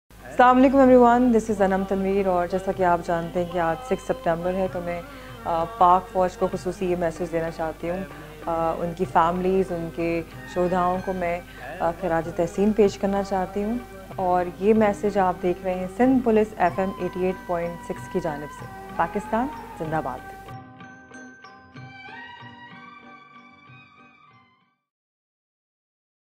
Anam Tanveer/ 6 SEP /DEFENCE DAY
The sacrifices paid by our courageous armed forces are unforgettable. I pay my tributes to our brave soldiers who sacrificed their lives in the line of duty.